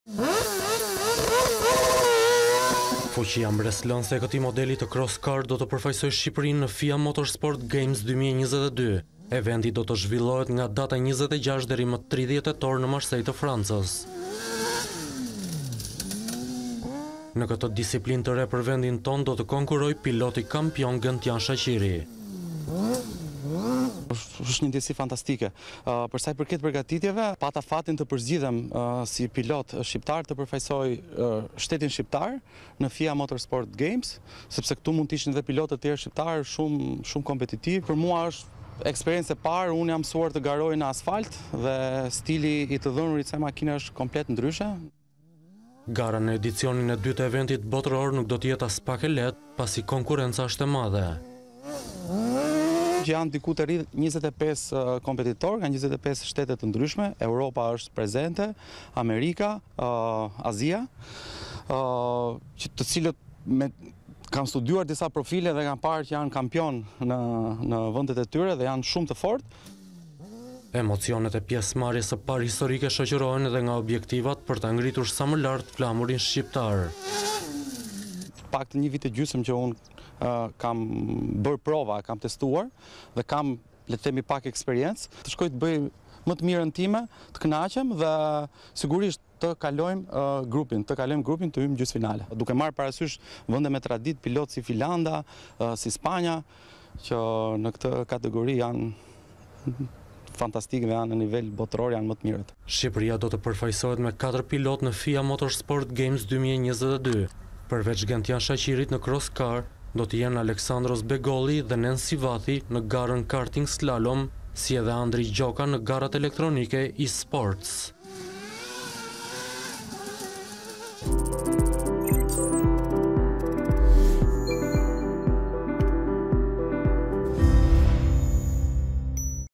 Shqipëria në Botërorin e Automobilizmit
Fuqia mbresëlënëse e makinës së Cross Car do të përfaqësojë Shqipërinë në FIA Motorsport Games 2022. Eventi do të zhvillohet nga 26 deri më 30 tetor në Marsejë të Francës. Në këtë disiplinë të re për vendin tonë do të konkurrojë piloti kampion Gentjan Shaqiri.